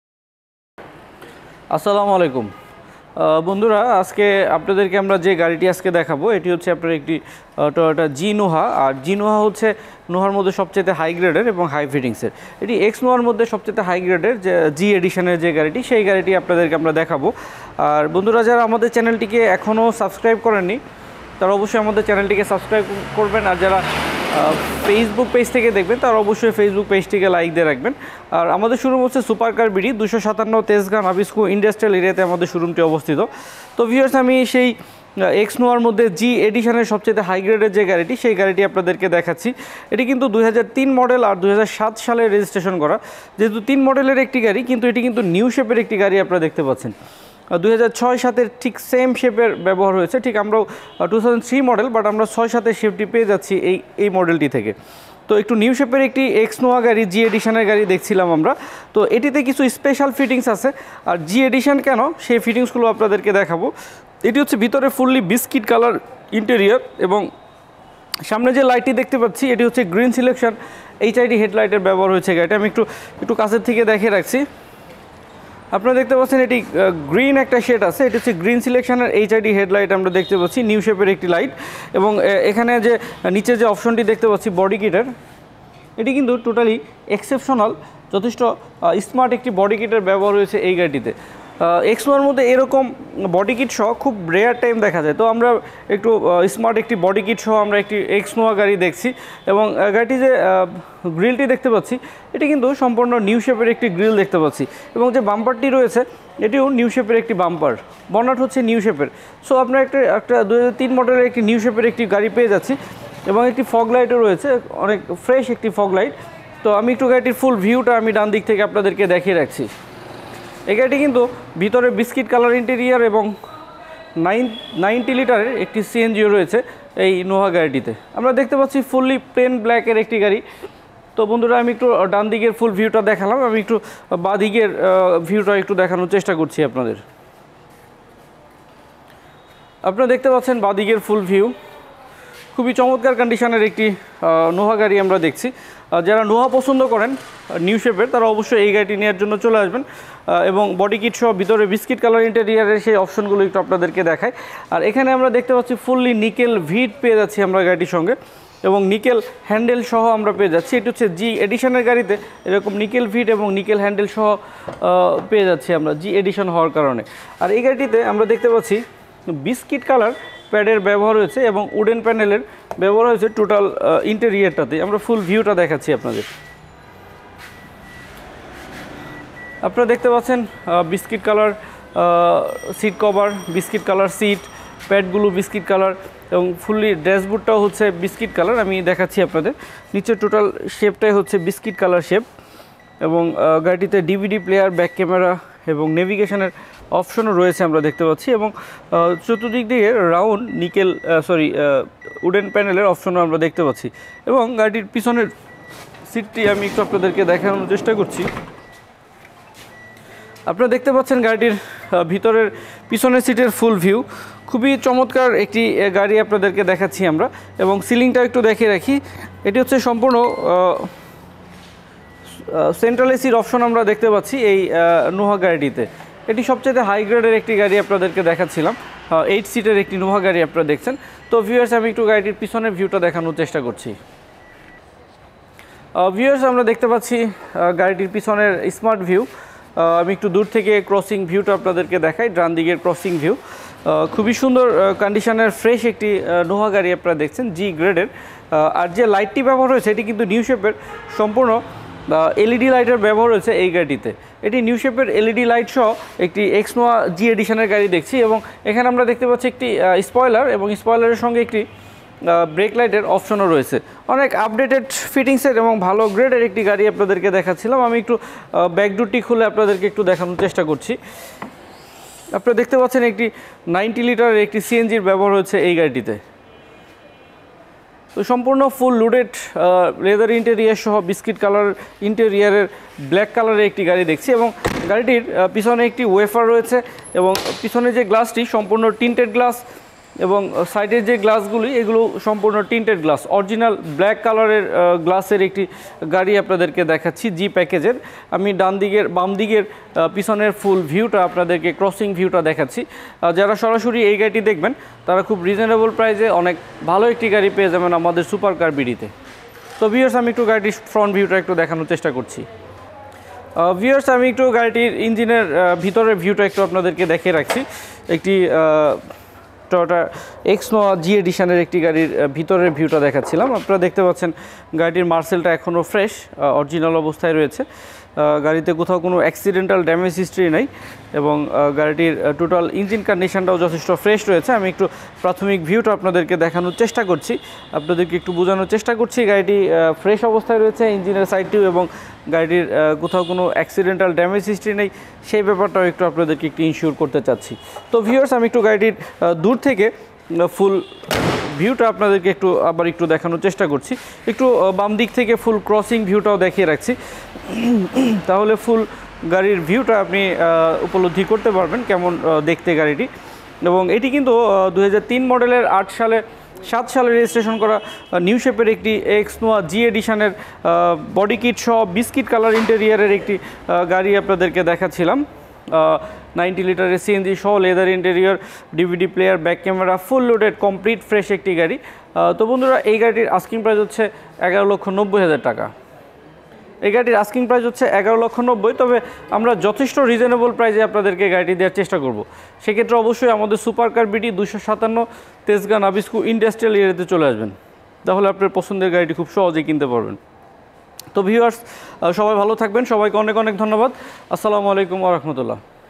Toyota Noah Review | Noah Price | Used Car Price In Bangladesh | Hiace | Micro Bus
Condition: Excellent
Car Name: Toyota X Noah
Model: 2003
Registration: 2007
Serial: 51
Engine: VVTI
Displacement: 2000 cc
Transmission: Automatic
Color: Black
Fuel System: Octane & Cng

Options: Excellent AC, Leather Interior, DVD Player, Back Camera, HID Headlight, Power Steering, Disk Brake, Power Windows & Mirror, ABS, Airbag, Central Lock, Tempered Glass, Alloy Rim, All Papers up to date, All Power.